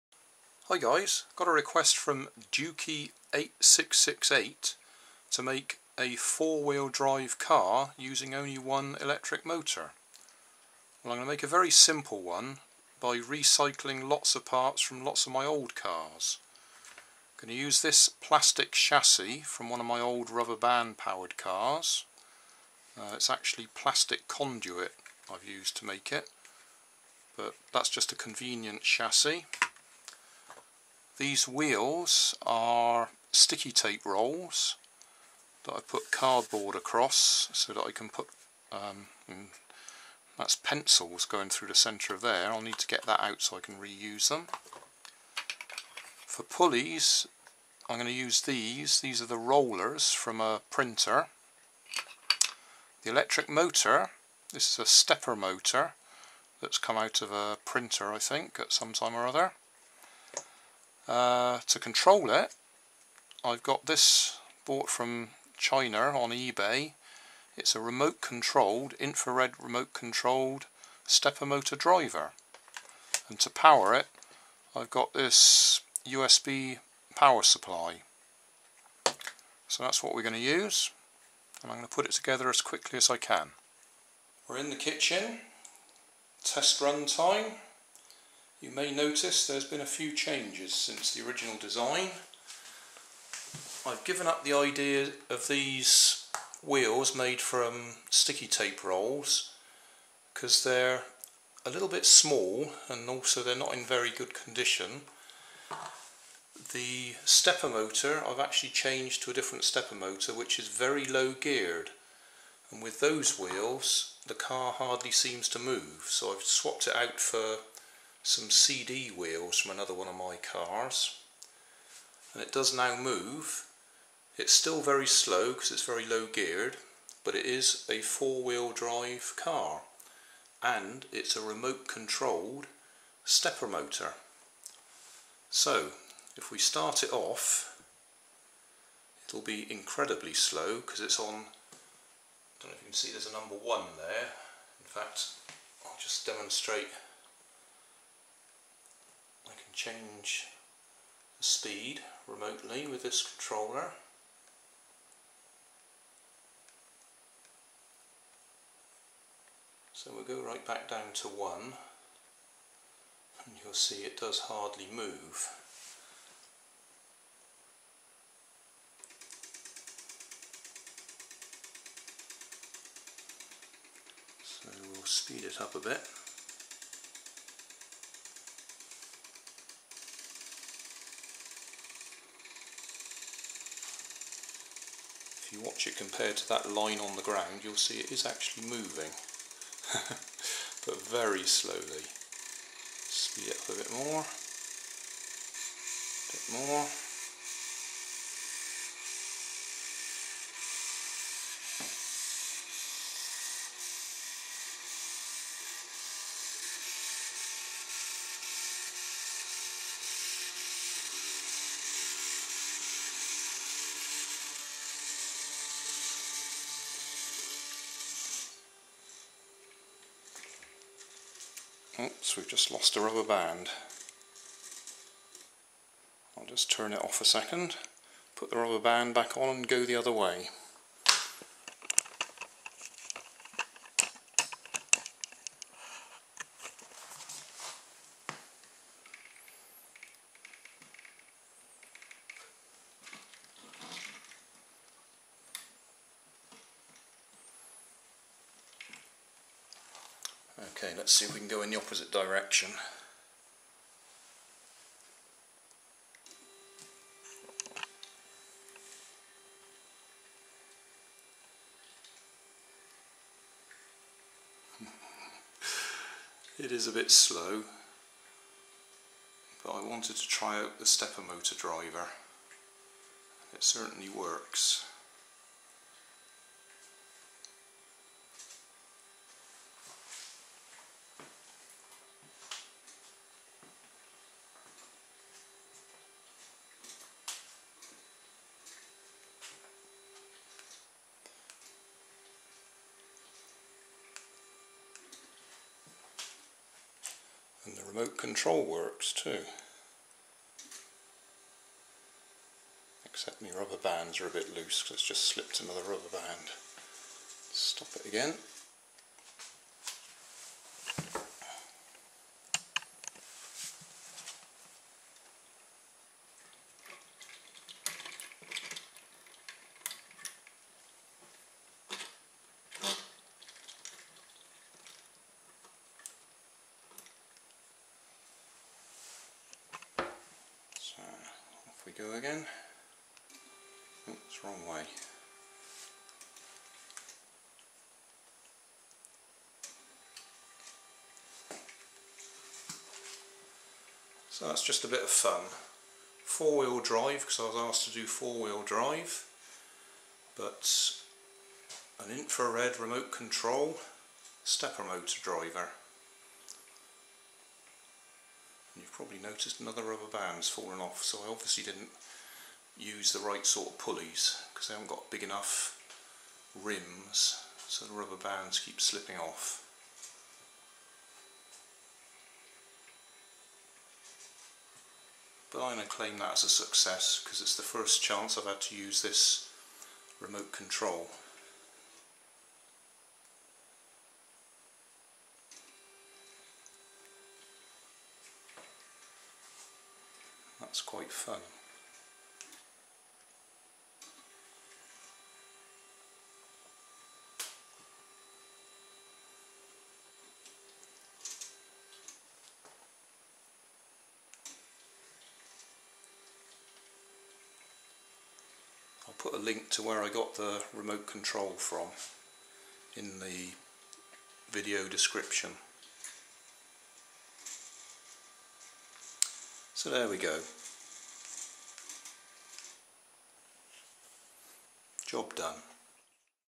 4 Wheel Drive Remote Control Stepper Motor Powered Car
Dukey8668 asked "Try to make a four wheel drive car with one electric motor." so it gave me an excuse to recycle lots of parts from some old projects and to try out my remote control stepper motor driver fresh from China via eBay.

eBay listing for the remote control stepper motor driver unit